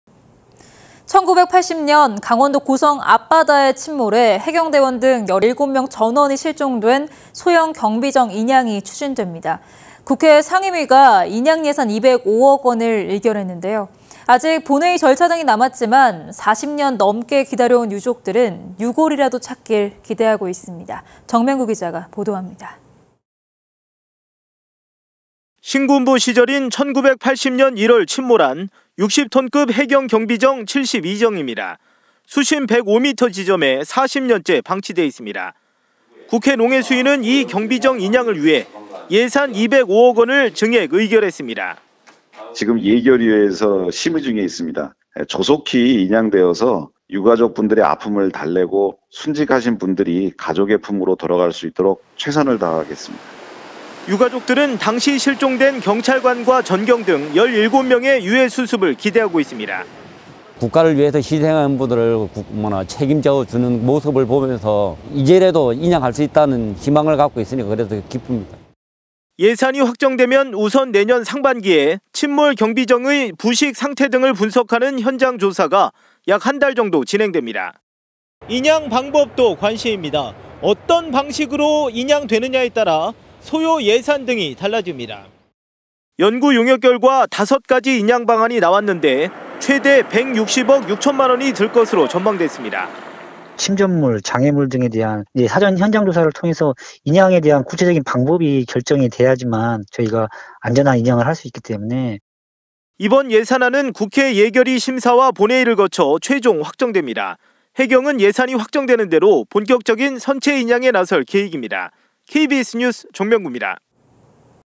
해경 경비정, 40년 만에 인양…205억 원 상임위 의결
[앵커]

 1980년,  고성 앞바다에 침몰해, 해경 대원 등 17명 전원이 실종된  소형 경비정 인양이 추진됩니다.

 국회 상임위가  인양 예산 205억 원을 의결했는데요.

 아직 본회의 절차 등이 남았지만,  40년 넘게 기다려온 유족들은 유골이라도 찾길 기대하고 있습니다.

 정면구 기자가 보도합니다.

 [리포트]

 신군부 시절인 1980년 1월 침몰한  60톤급 '해경 경비정 72정'입니다.

 수심 105미터 지점에 40년째 방치돼 있습니다.

 국회 농해수위는 이 경비정 인양을 위해  예산 205억 원을 증액 의결했습니다.

  [이양수/의원/국회 농해수위 : "지금 예결위에서 심의 중에 있습니다. 조속히 인양되어서 유가족분들의 아픔을 달래고 순직하신 분들이 가족 품으로 돌아갈 수 있도록 최선을 다하겠습니다."]

 유가족들은  당시 실종된 경찰관과 전경 등 17명의 유해 수습을 기대하고 있습니다.

 [조병주/해경 72정 유가족협의회 대표 : "국가를 위해 희생한 분을 책임져주는 모습을 보면서 이제라도 인양할 수 있다는 희망을 갖고 있으니 그래도 기쁩니다."]

 예산이 확정되면 우선 내년 상반기에  침몰 경비정의 부식 상태 등을 분석하는 현장조사가 약 한 달 정도 진행됩니다.

 인양 방법도 관심입니다.

 어떤 방식으로 인양되느냐에 따라, 소요 예산 등도 달라집니다.

 연구용역 결과  5가지 인양 방안이 나왔는데,  최대 160억 6천만 원이 들 것으로  전망됐습니다.

 [제강용/동해지방해양경찰청 수색구조계장 : "(72정 주변) 침전물, 장애물 등에 대한 사전 현장 조사를 통해 인양에 대한 구체적 방법이 결정돼야지만 저희가 안전한 인양을 할 수 있기 때문에."]

 이번 예산안은 국회 예결위 심사와 본회의를 거쳐 최종 확정됩니다.

 해경은 예산이 확정되는 대로  본격적인 선체 인양에 나설 계획입니다.

 KBS 뉴스 정면구입니다.